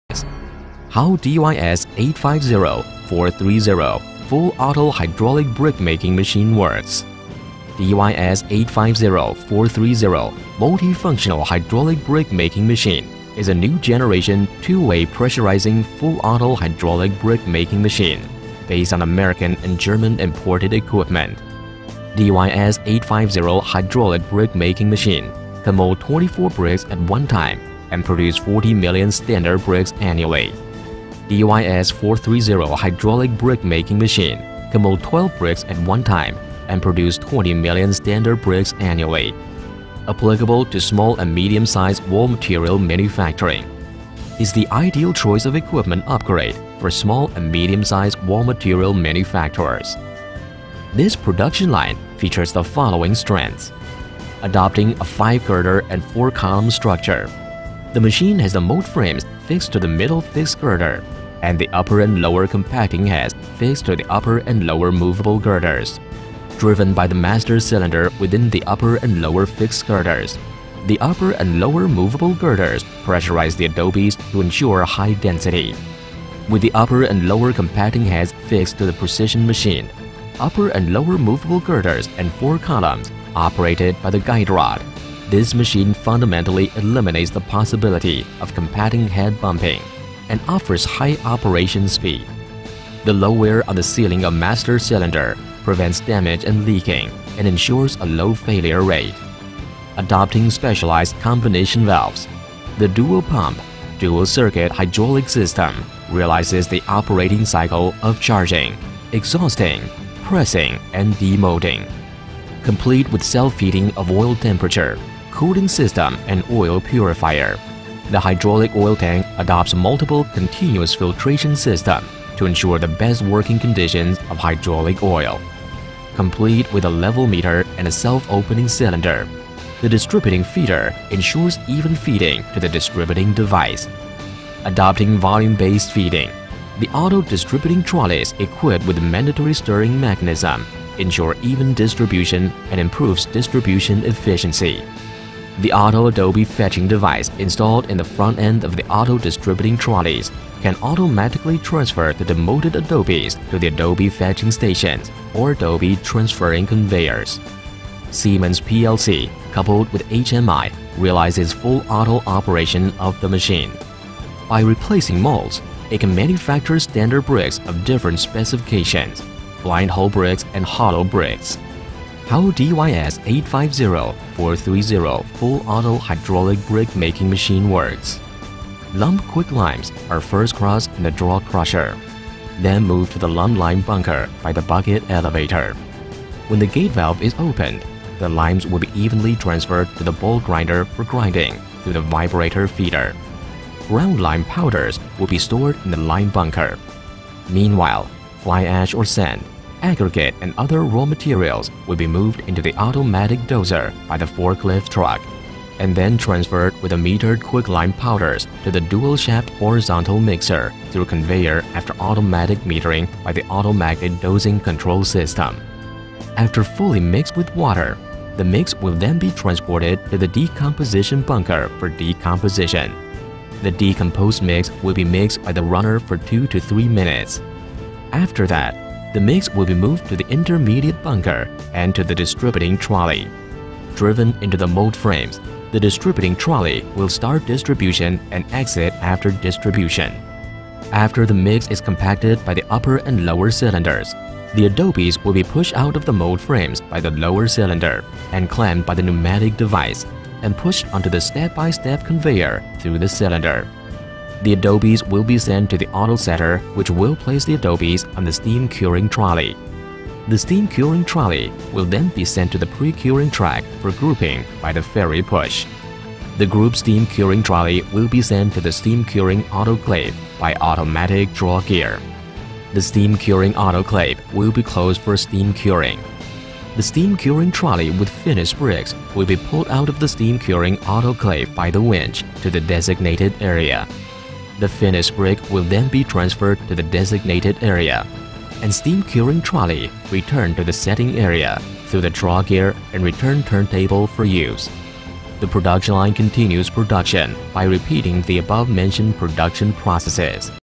DY850 430 Hydraulic Brick Making Machine
Based on digesting imported equipments from US and Germany, we researched and developed the multi-functional automatic hydraulic brick making machine after repeated tests, DYS 850/430 is a new-generation of double-way press automatic brick making machine. It has been verified by the experts of Henan Technology Department. In addition, the product has been awarded many national patents.

Taking sand, fly ash, gangue, slag, quarry, dust, debris, ceramic aggregate, building waste and cement as materials, the brick-making machine, by replacing of different moulds and simple screen touching, is able to produce standard bricks, hollow bricks, blind hole bricks, square bricks and pavement bricks and other bricks for new-style industrial building and living.